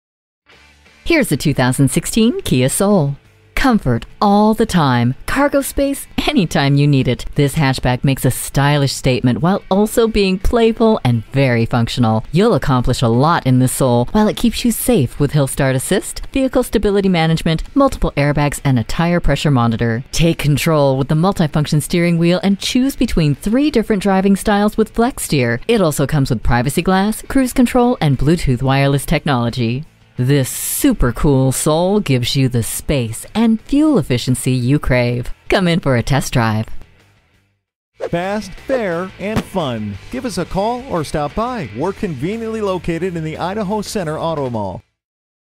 2016 Kia Soul Boise ID Nampa, ID #960126 - SOLD
Year: 2016
Make: Kia
Model: Soul
Trim: Base
Engine: 4 Cyl. 1.6 L
Transmission: Automatic
Color: Clear White
Interior: Black
Stock #: 960126
VIN: KNDJN2A23G7822083
C Kia has done it again! They have built some terrific vehicles and this good-looking 2016 Kia Soul is no exception! It will take you where you need to go every time...all you have to do is steer! This Soul is nicely equipped.

Address:
15700 Idaho Center Blvd